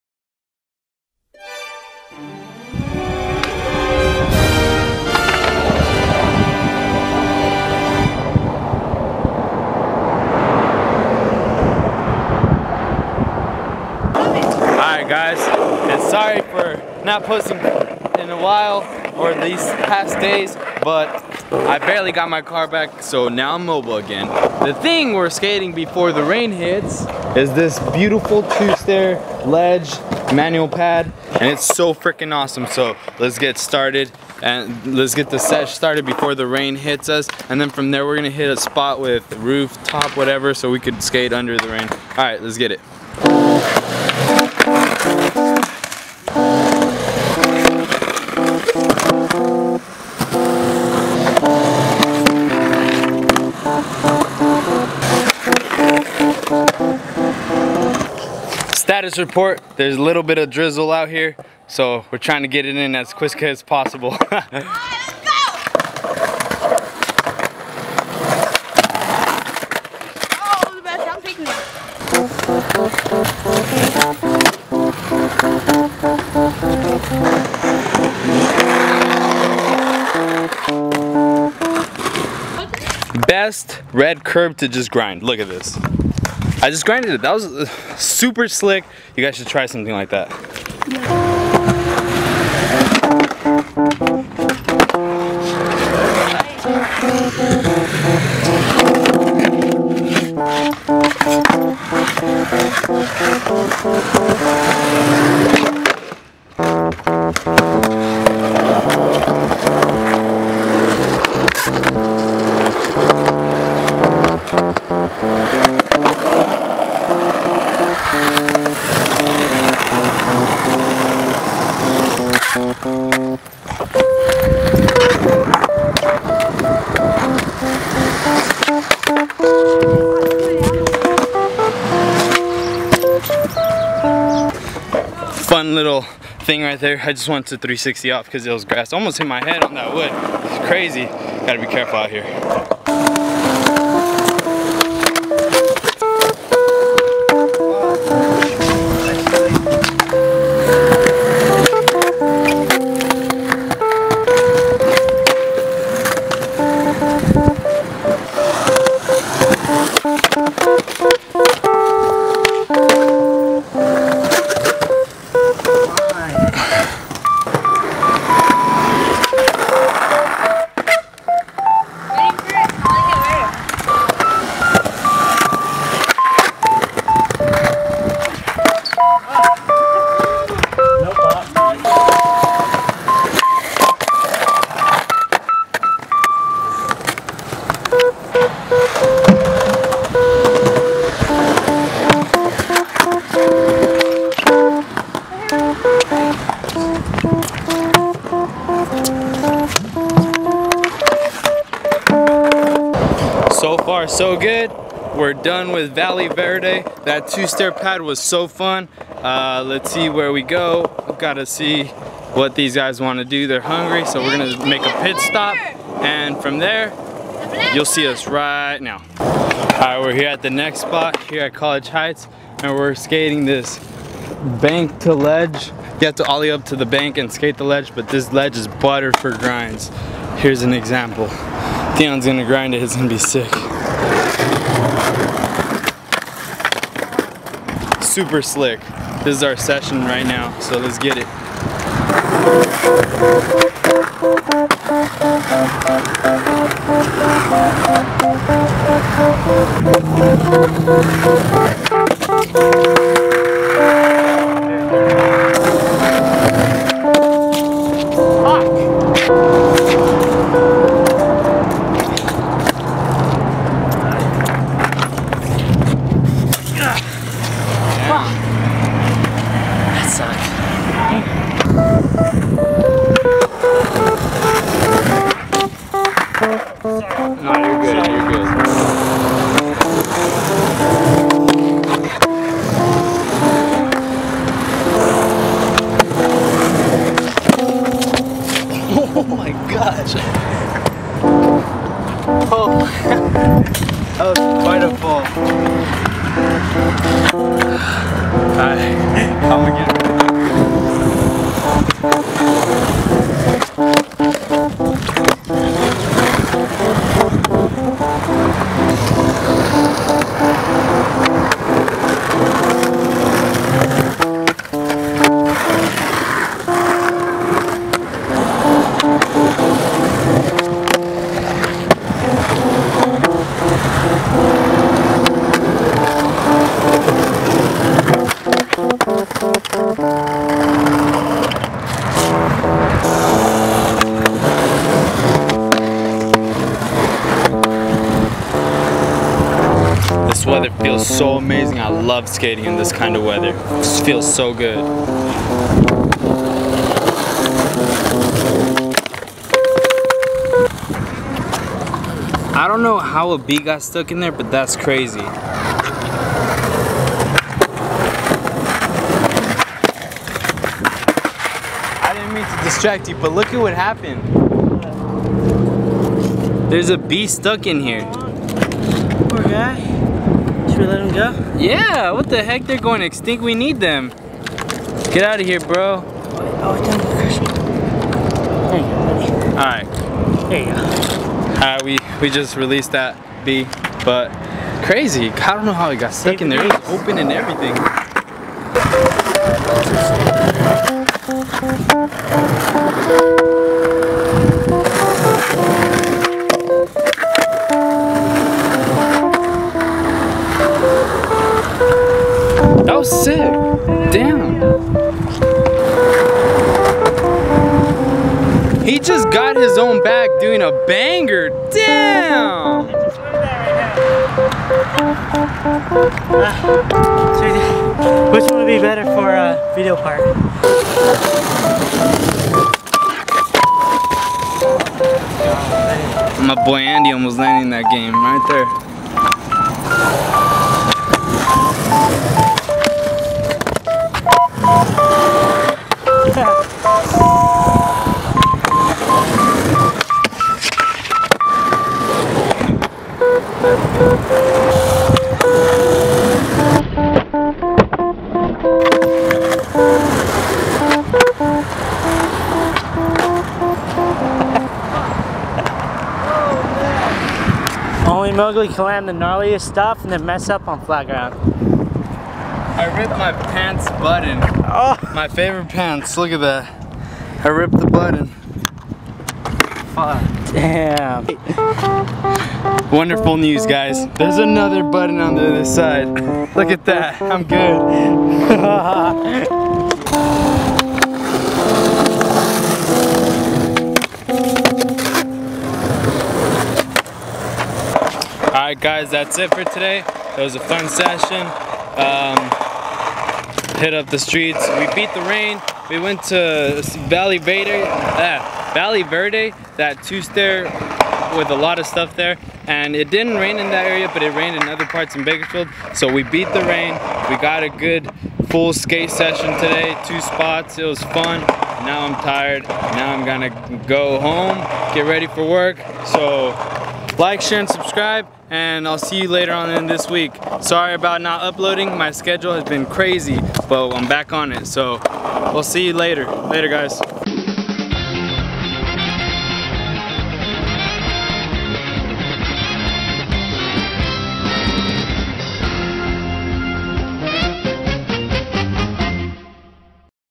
Two Spots No Rain!
So hyped Back with my car so I'm able to show you guys diff spots! in this video the weather here in bakersfield was supposed to rain but it rained in patches so we beat it Enjoy.